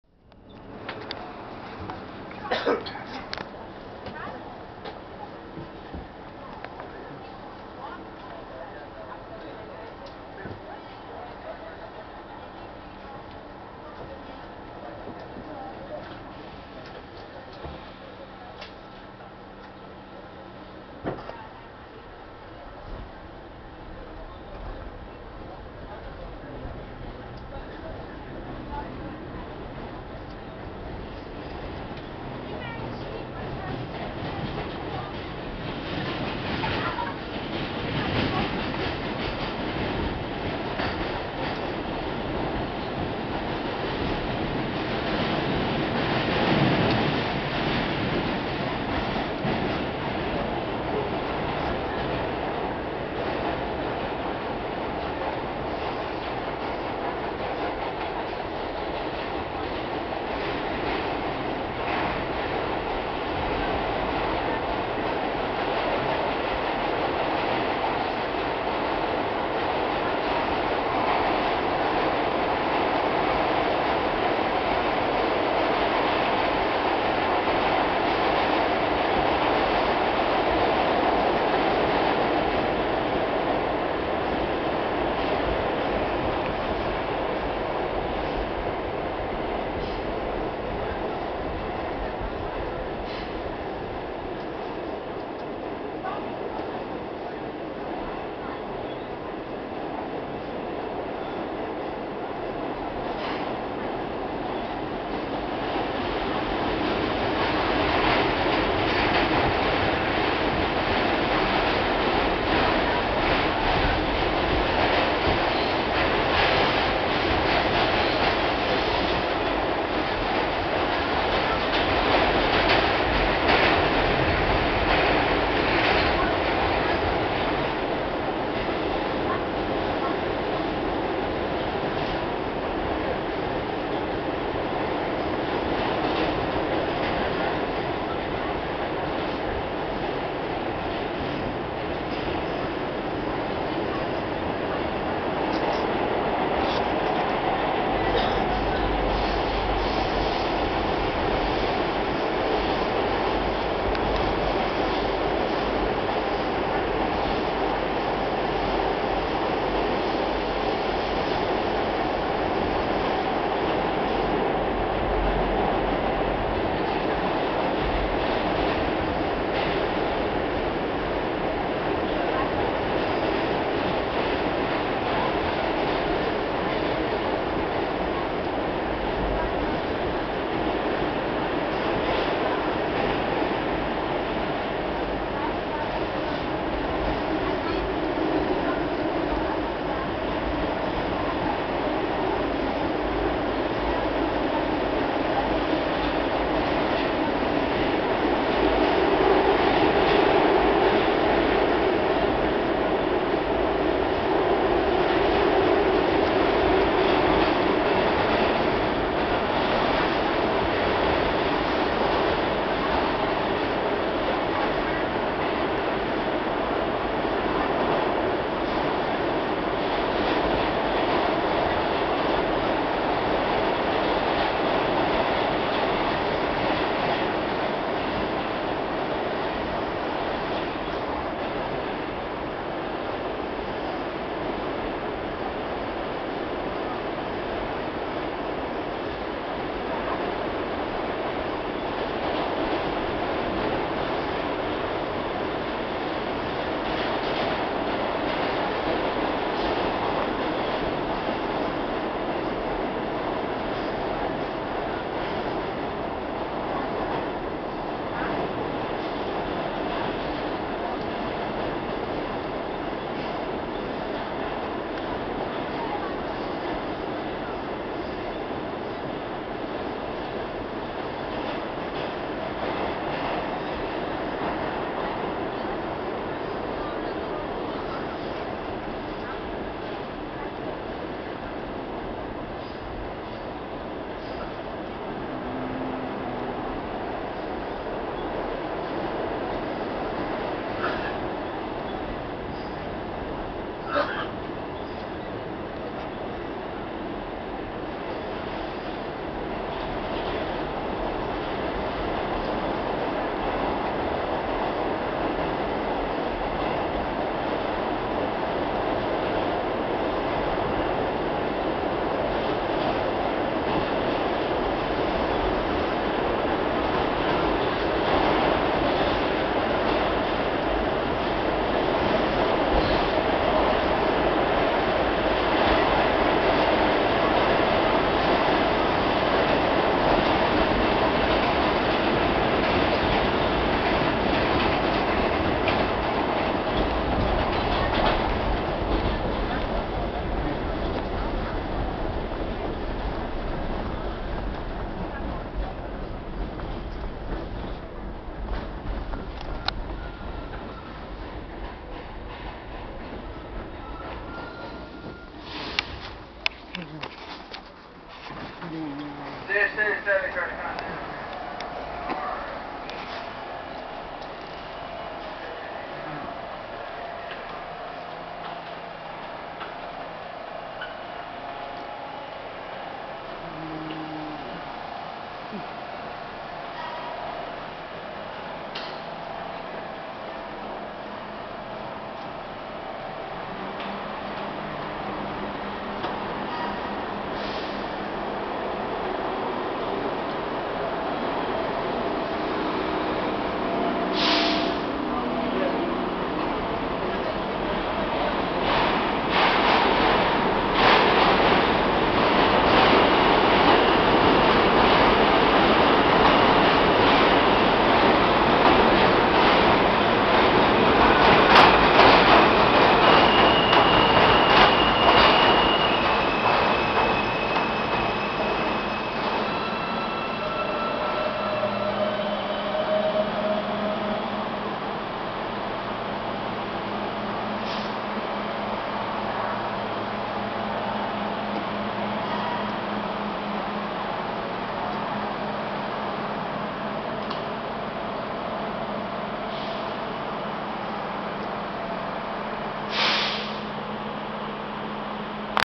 3. MTA New York City Subway R-46 F Train Jackson Hts-Roosevelt Ave to Forest Hills-71 Ave
R-42 R train here at Forest Hills-71 Ave